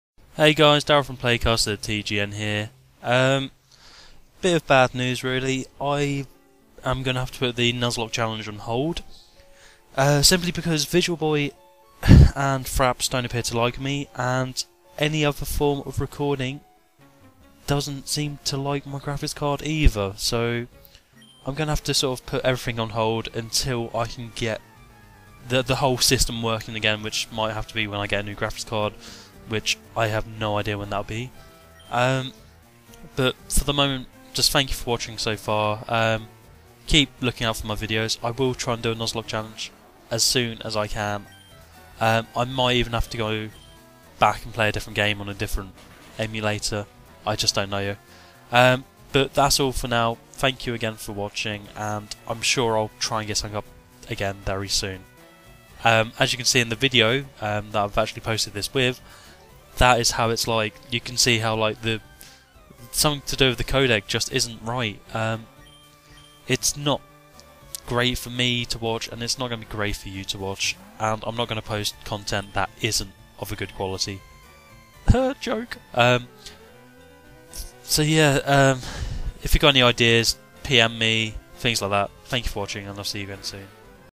★ Nuzlocke Challenge: Pokemon Fire Red - Episode 9.5 HIATUS
Hey guys, just an update on my Pokemon Nuzlocke challenge series. Basically any form of recording appears to break my display/graphics settings and I don't want to upload bad looking content - so it'll be on a hiatus til I can get it fixed.

Nuzlocke Challenge Rules
- Any Pokémon that faints is considered dead, and must be released.
- The player may only catch the first Pokémon encountered in each area, and none else. If the first Pokémon encountered faints or flees, there are no second chances.
- Optional: Pokemon must be renamed as they are captured.

How do I get more views on YouTube?